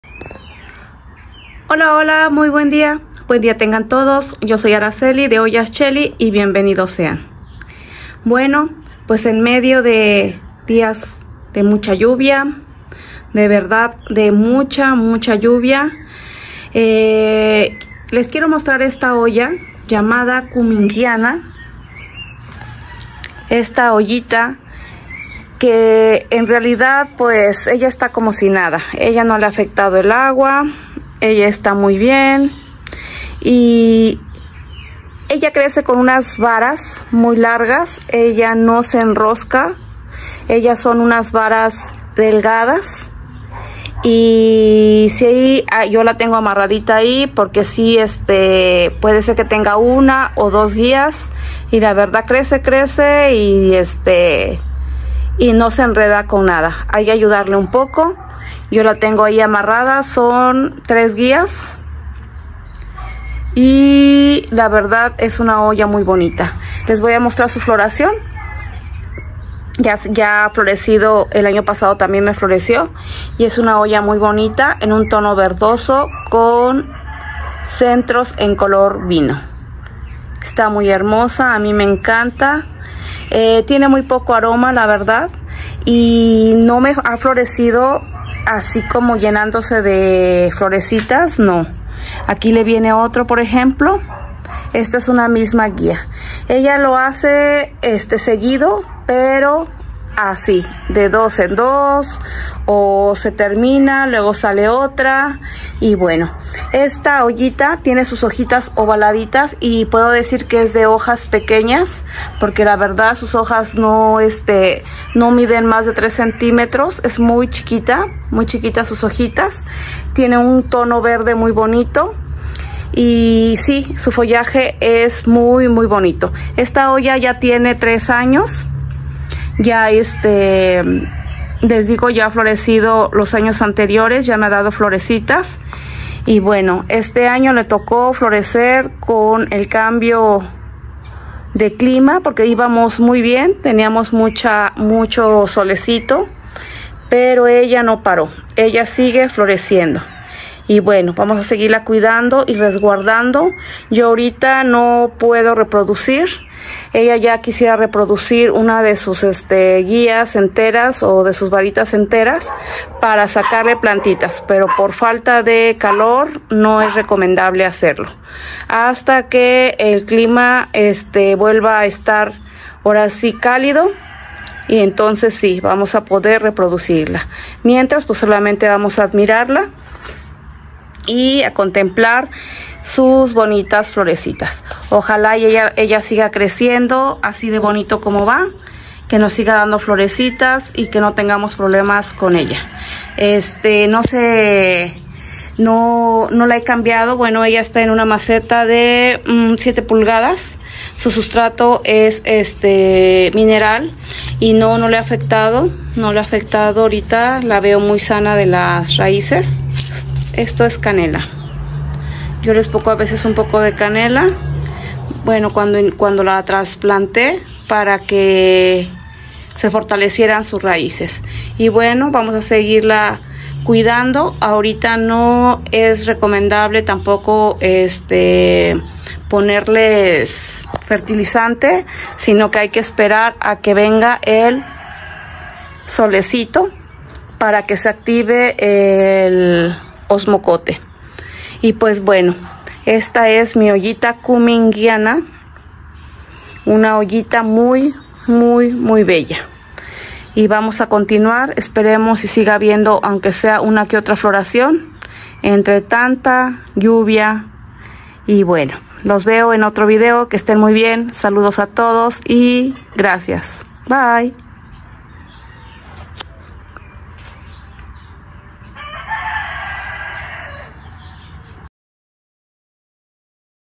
🌼Hoya Cumingiana🌼Una flor con colores que contrastan.🌼Flor de cera.🌼
A pesar de tanta agua y exceso de humedad,floreció mi hoya cumingiana, me encanta su combinación de tonos y forma de sus flores.🥰